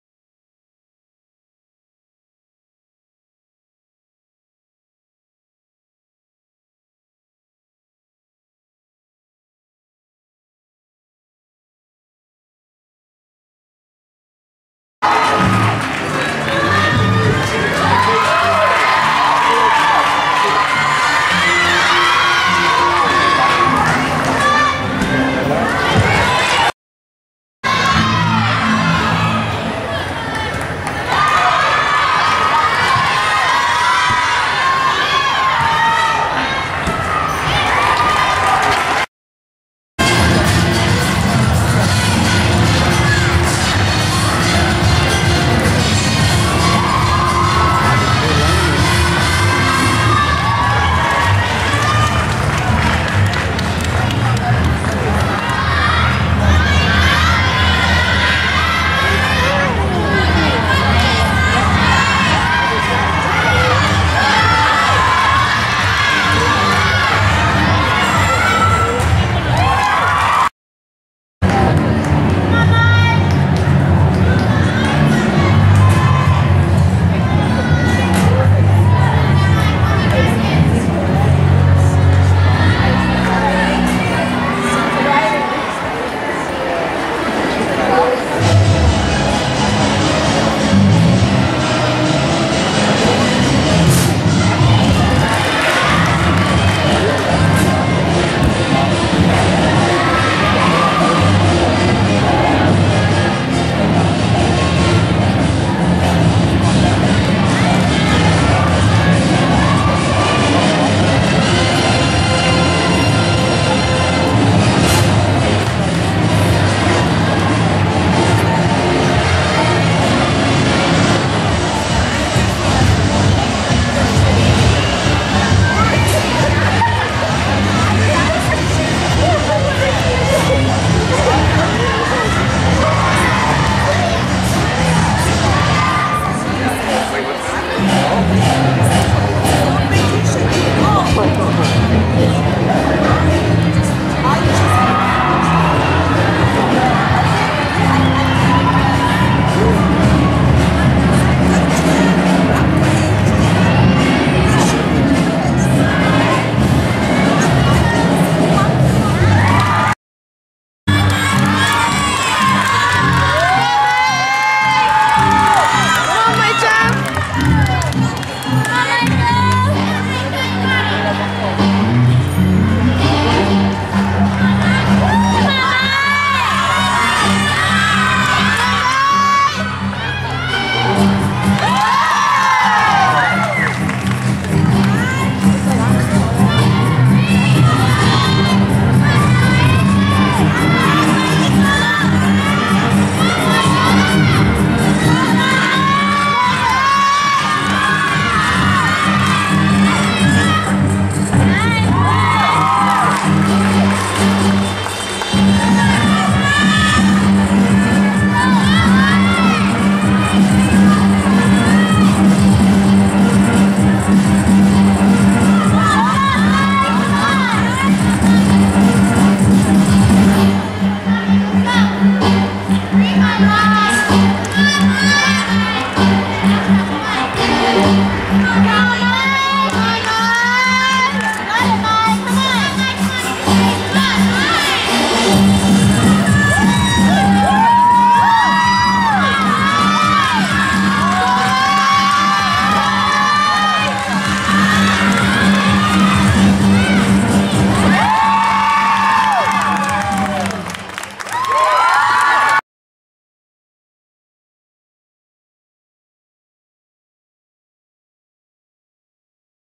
Maiko Suarez lvl 10 Lady Luck 2013.wmv
Maiko Suarez Level 10 Class of 2013
Las Vegas Lady Luck Invitational